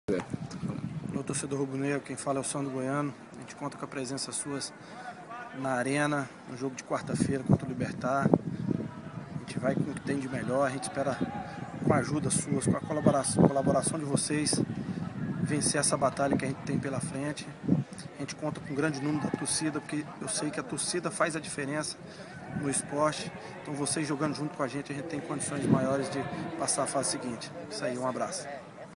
Sandro Goiano convoca a torcida do Sport para a partida contra o Libertad
O ex-atleta do Sport  e atual Coordenador Técnico de Futebol do clube, Sandro Goiano, convoca a torcida rubro-negra para lotar todos os setores da Arena Pernambuco, na partida da quarta-feira (23/10/2013), contra o Libertad, do Paraguai, pela Copa Sul-Americana.